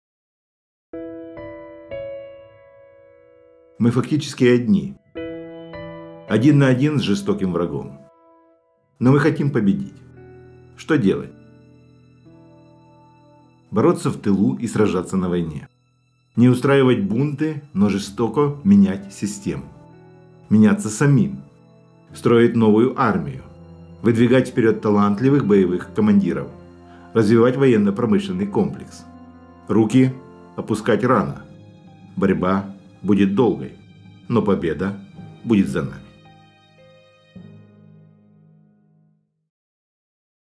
Как украинцам победить в этой войне?
Простой рецепт победы в противоборстве с кремлевской системой.